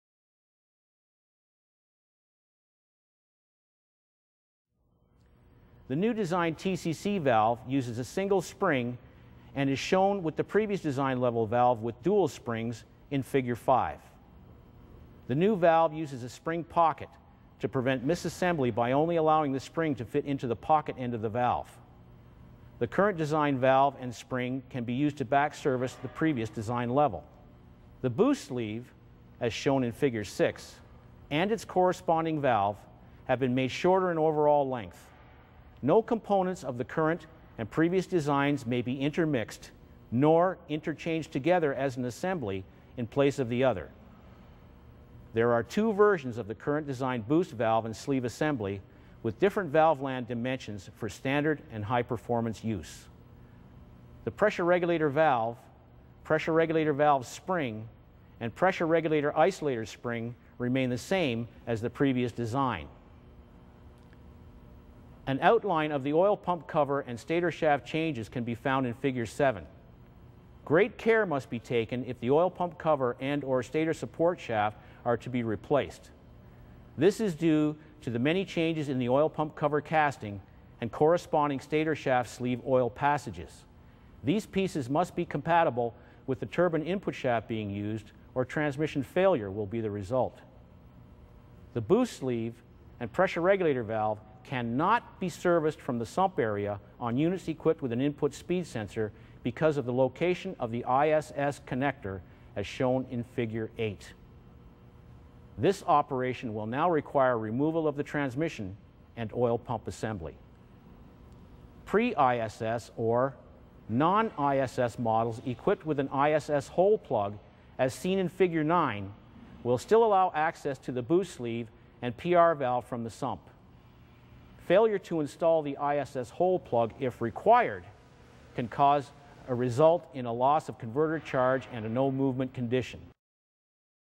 Automatic Transmission: How to Repair a 2006 Turbine Input Shaft Speed Sensor 4L60E Part 2
- Automatic Transmission: How to Repair a 2006 Turbine Input Shaft Speed Sensor 4L60E, 4L65E & 4L70E Part 2 with Gerald Campbell. This series is a continuation on the 2006 Turbine Input Shaft Speed Sensor which has been added to the 4L60E, 4L65E & 4L70E series transmissions in limited S & T series trucks with Gerald Campbell from ATSG!

Described and explained by Gerald Campbell, Sr. Technician at Automatic Transmission Service Group ATSG

and join today!

automatic transmission
transmission problems
transmission problem
automatic transmissions
transmissions